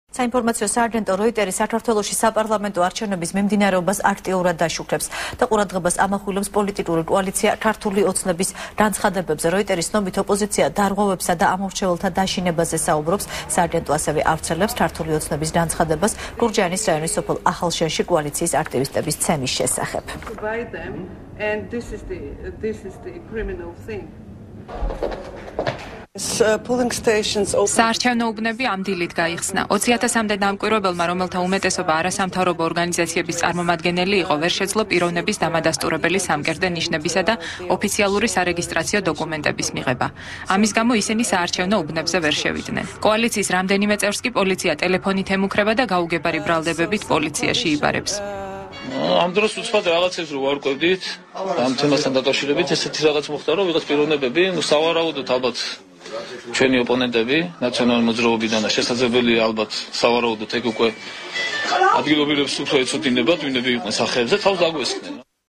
ახალი 4 | როიტერი დარღვევებზე | 01.10.12
როიტერი საპარლამენტო არჩევნებს აქტიურად აშუქებს